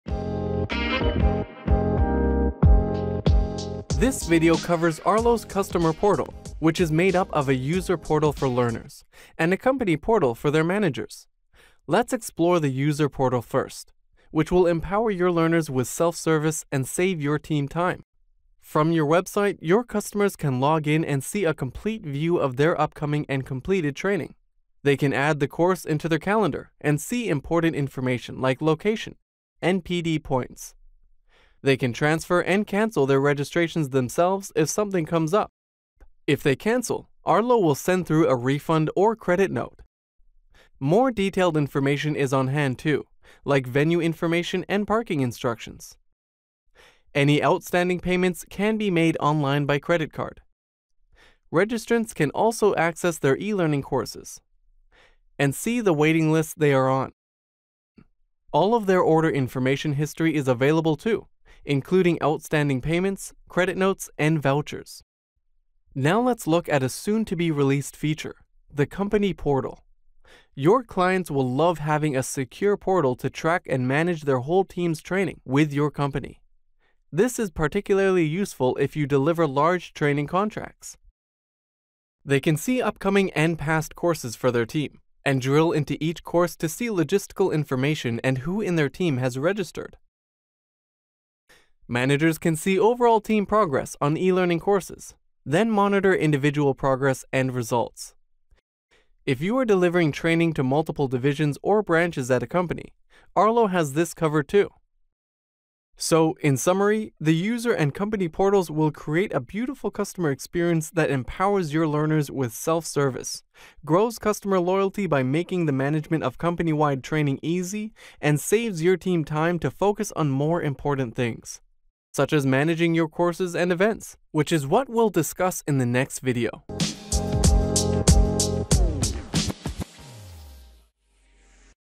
Customer portal | Arlo Training Management Software
This video covers Arlo’s Customer portal which will empower your learners with self-service, saving your team time and growing customer loyalty.